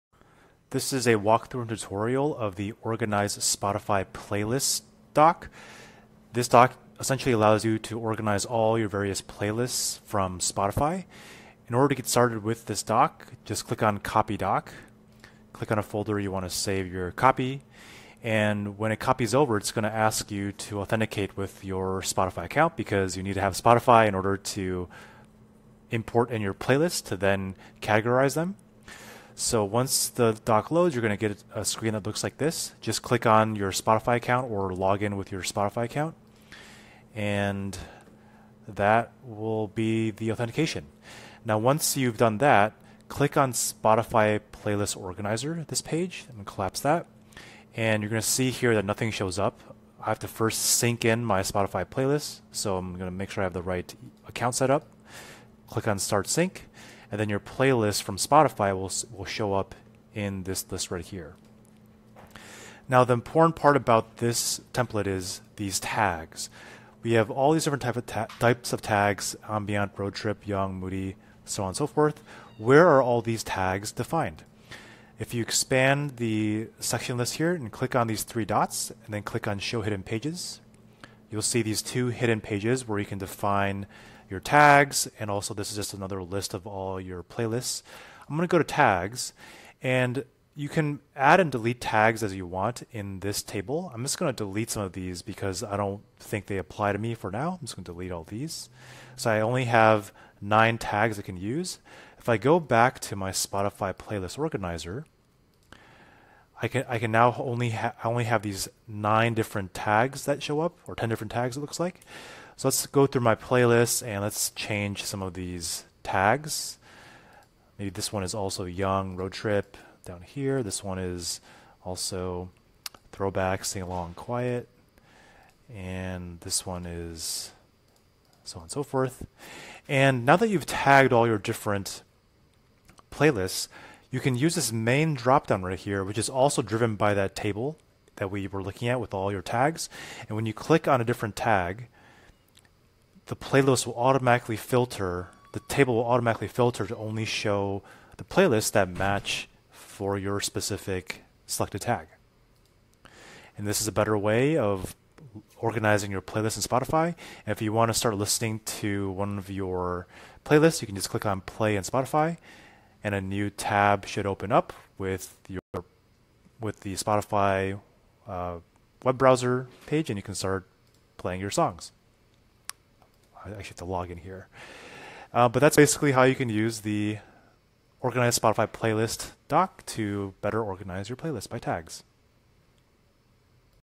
How to Organize Spotify Playlists - Template & Tutorial (Coda)
This is a tutorial on how to organize your Spotify playlists with tags in Coda

Easily jump to sections in this video:
* Intro - 0:00
* Copy the doc - 0:14
* Login with Spotify - 0:32
* Sync your Spotify playlists - 0:56
* Unhide hidden pages - 1:18
* Customize your playlist tags - 1:28
* Tag each of your playlists - 1:47
* Filter playlists by a tag - 2:16
* Listen to your Spotify playlist - 3:03

If you have many Spotify playlists, categorizing your playlists can become difficult in Spotify. This Coda doc allows you to tag your playlists with tags you define so you can quickly sort through all your playlists.

Perhaps you name your playlists by season, genre, mood, and more. By customizing your own playlist tags in this doc, you can find the playlists you want to listen to in a snap. This solves a few problems: 1) playlists can belong to multiple categories 2) older playlists in Spotify don't get played as much 3) can't share your collection of playlists with friends.

Features in this template:
✅ Tag a playlist with multiple tags
✅ Filter playlists by any tag and listen to playlist with a click of a button